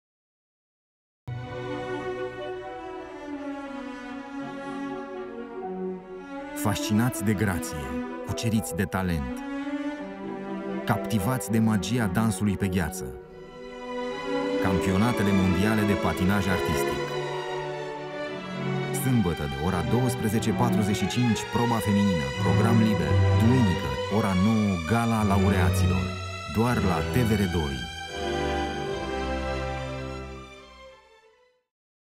Weekend cu patinaj artistic la TVR2
Sâmbătă, 29 martie - de la ora 12:45 - program liber, feminin;

Duminică, 30 martie - de la ora 09:00 - Gala laureaţilor;